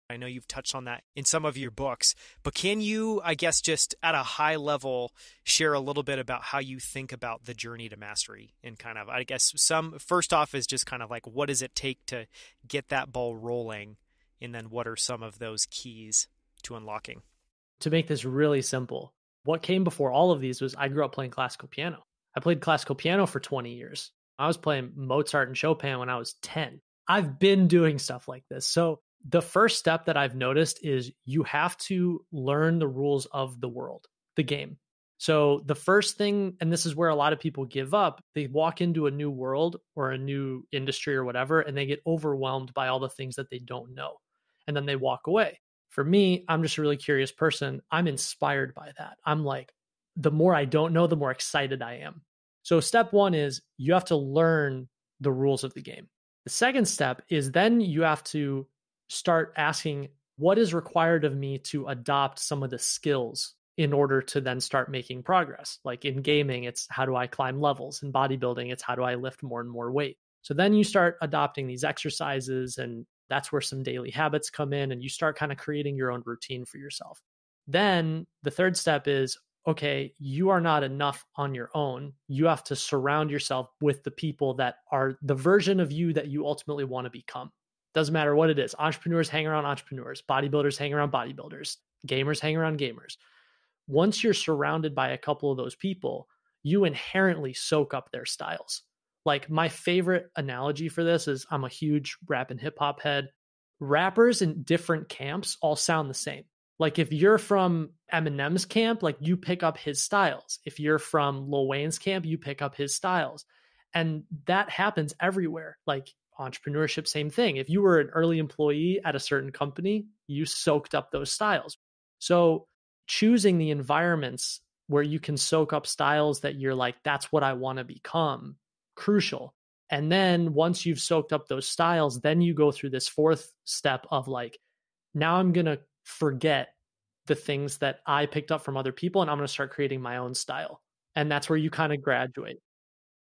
Nicolas Cole – Three Steps to Mastery of Any Skill
“You have to surround yourself with the people that are a version of who you ultimately want to become.” – Nicolas Cole

To date, Nicolas Cole’s (@Nicolascole77) writing has racked up more than 100 million views. His work has appeared in Harvard Business Review, TIME, Forbes, Fortune, Business Insider, The Chicago Tribune, Apple News, Slate Magazine, Inc Magazine, and on CNBC. While he’s focused more on writing books, he started his career on Quora, of all places—where he’s been named a Top Writer four different times.

Nicolas is also the Founder and CEO of Digital Press, where he’s helped over 300 leaders create great content and, in many cases, publish their first book. Digital Press’s clients include NYT best-selling authors, Olympic athletes, award-winning musicians, Silicon Valley venture capitalists, and founders of some of the fastest-growing companies in the world.

His latest book, The Art and Business of Online Writing: How to Beat the Game of Capturing and Keeping Attention, is an incredible primer to writing online. It includes a slew of counterintuitive tips, tricks, and ideas for finding your voice and building an audience in record time.

Full show notes with selected links, quotes, and the big ideas in this episode:

If you enjoyed this podcast, please leave a short review in Apple Podcasts.